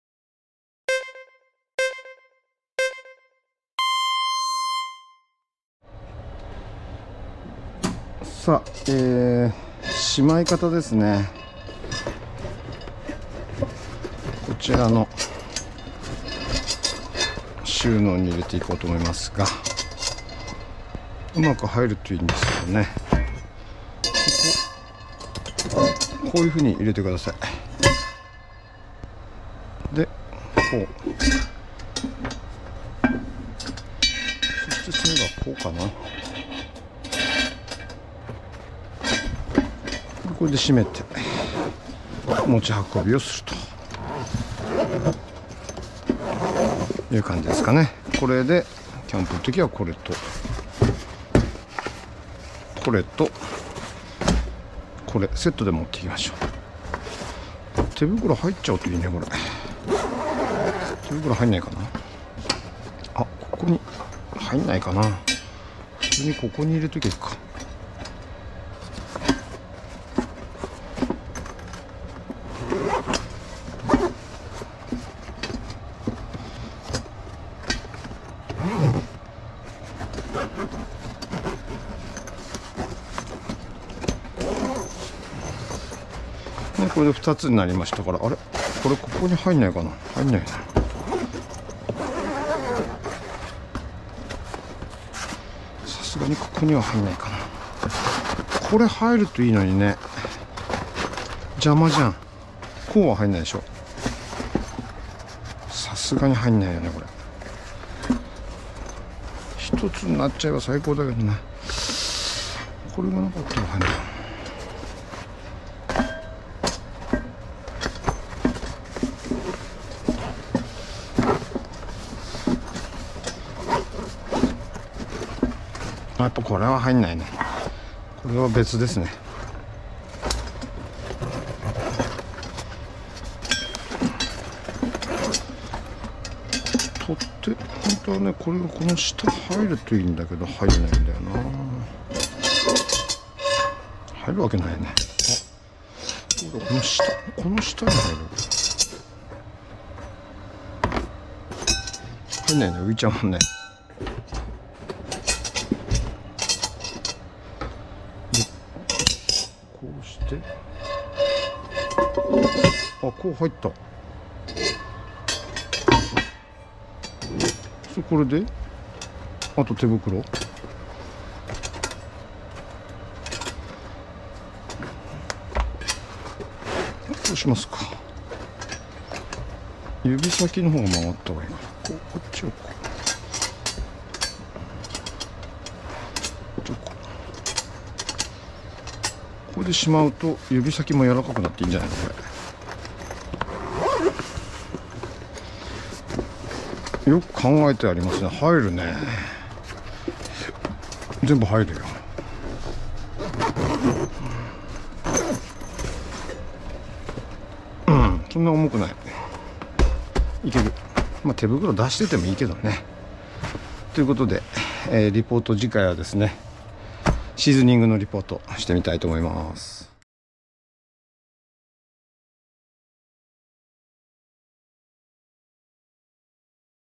かまど小屋ー和鉄ダッジオーブン収納ケースM
スノーピーク製のダッジオーブンを収納するケースのご紹介

中々丈夫でいい感じです